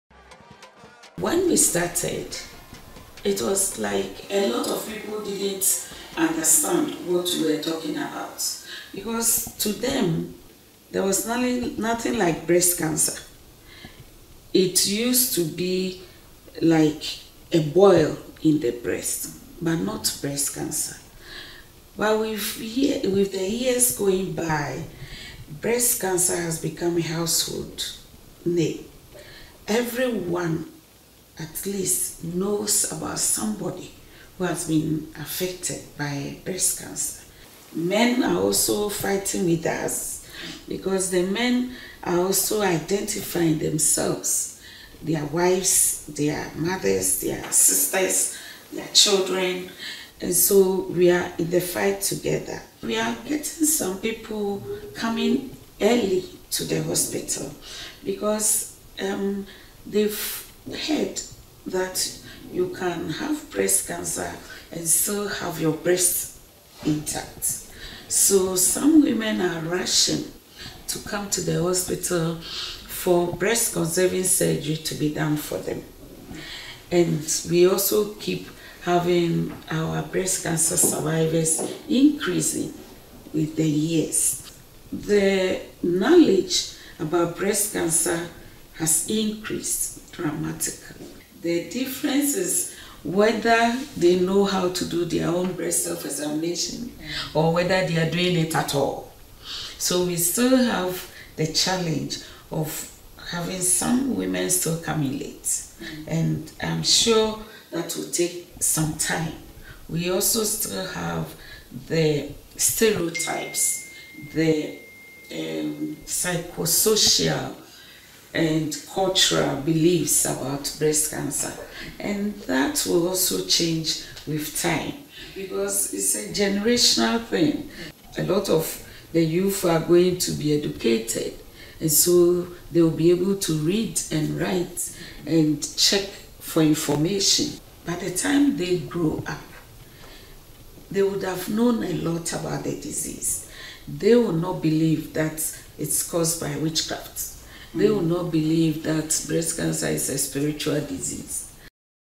Ghanaian Breast Cancer Expert Speaks on Progress in the Fight Against the Disease
Breast cancer is the most common cancer in women globally. The World Health Organization says there were over 2 million new cases of breast cancer in 2018. More than 60 percent of all breast cancer deaths occur in developing countries. Experts say late diagnosis is a serious challenge in the fight against breast cancer in sub-Saharan Africa. VOA health correspondent Linord Moudou spoke earlier this year with Dr. Beatrice Wiafe-Addai, a Ghanaian breast surgeon, chairperson of the Ghana Cancer Board, and founder of Breast Care International, an organization working to fight breast cancer in Ghana and lower the incidence of late-stage disease and diagnosis. She elaborates on progress made in the fight against the disease in Ghana and some remaining challenges.